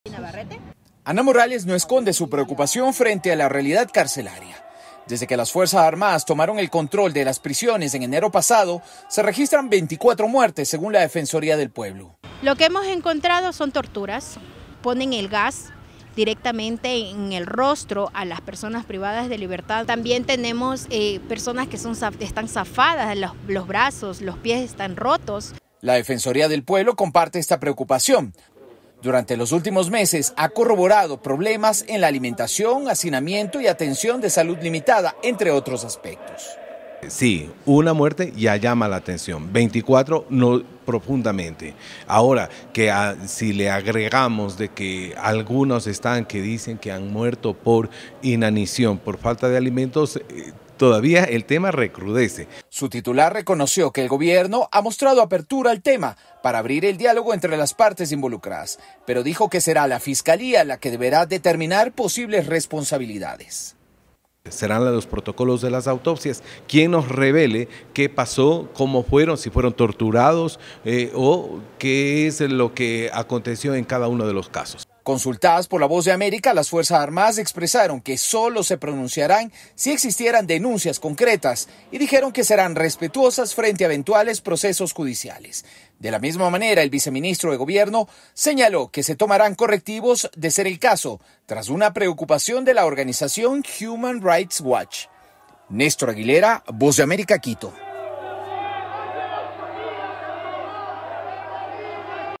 Familiares de presos ecuatorianos denuncian violaciones de derechos humanos
A pesar del control de las Fuerzas Armadas en el interior de las cárceles ecuatorianas, familiares de reos denuncian nuevas muertes. Y si bien la cifra es baja respecto a los crímenes ocurridos en recientes masacres, hay preocupación en la Defensoría del Pueblo. Desde Quito, Néstor Aguilera, de la Voz de América.